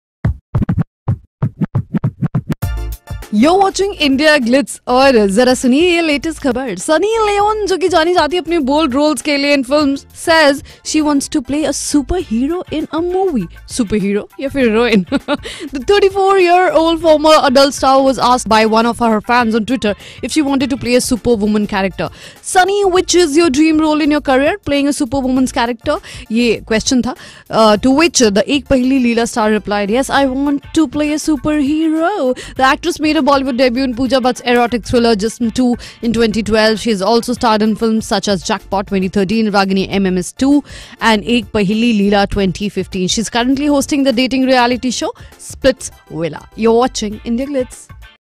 Sexy Sunny Leone's Dream To Be Superhero | Splitsvilla, Ek Paheli Leela, Ragini MMS 2
Actress Sunny Leone has expressed her desire to play a superhero in a movie.